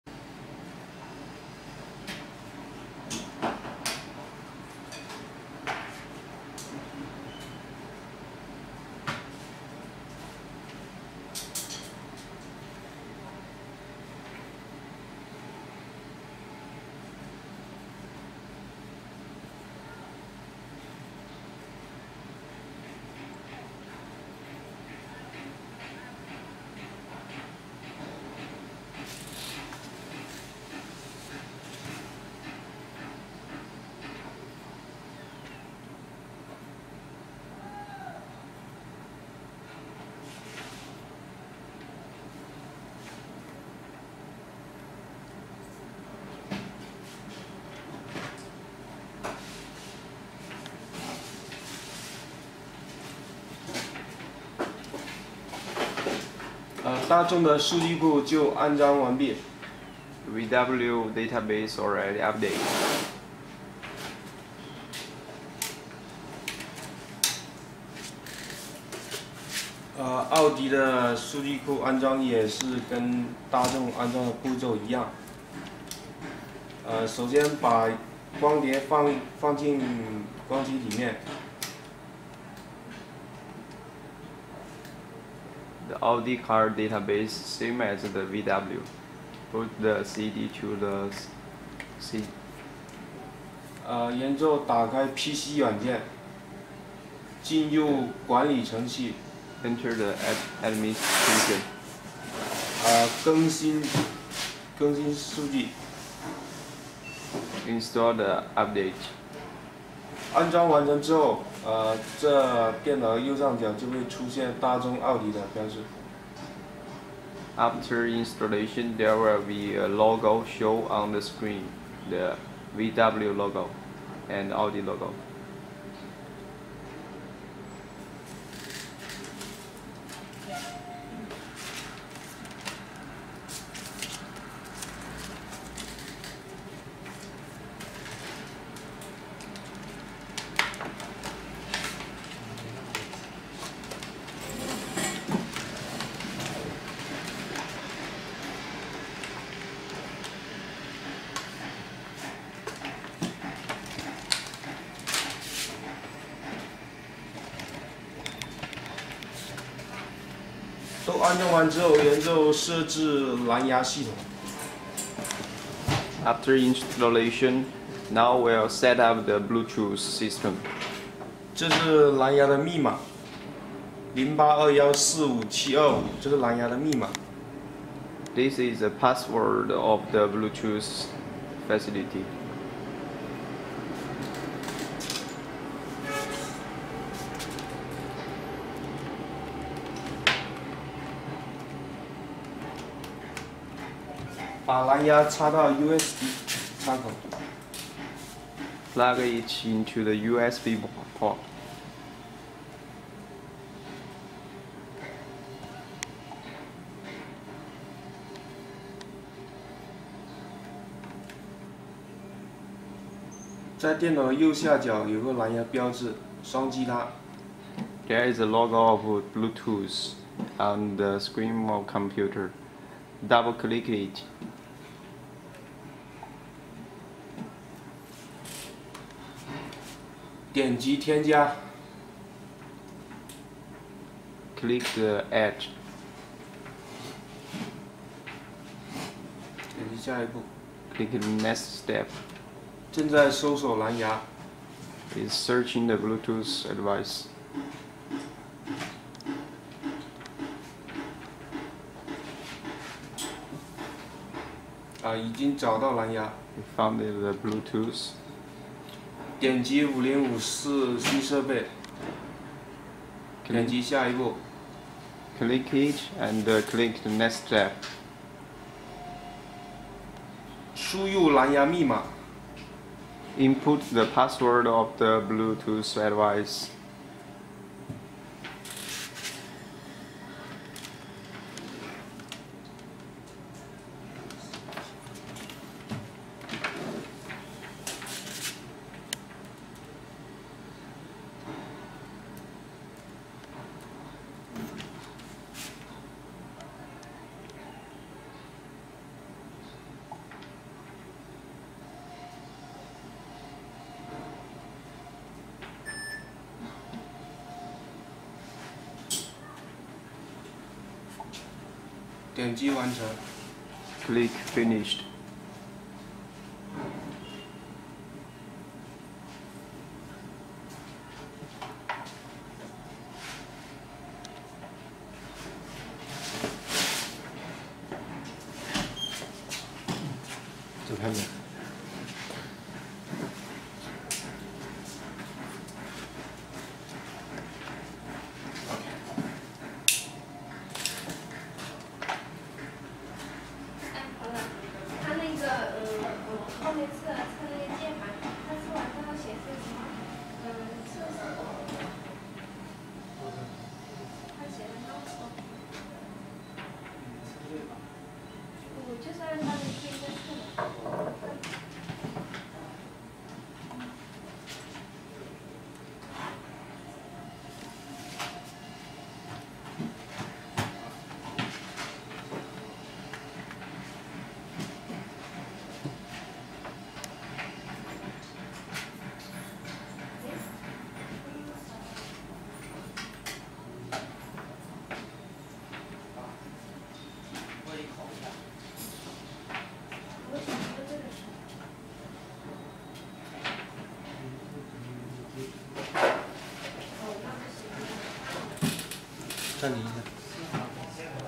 VAS 5054A VW Audi Bentley and Lamborghini Multi-language diagnostic tool video
The VAS 5054A is a universal diagnostic interface for the vehicles of the Volkswagen Group and all OBD vehicle systems from other manufacturers.
The integrated Bluetooth interface connects the vehicle with a notebook, desktop PC or test system.
No cables are needed for vehicle diagnostics using the VAS 5054A.
The range of the wireless Bluetooth connection is 5 to 10 meters,depending on the environment and the type of Bluetooth adapter used on the PC side.
The Serial Port Profile (SPP) is used for data transfer via Bluetooth. Different Bluetooth security levels are supported.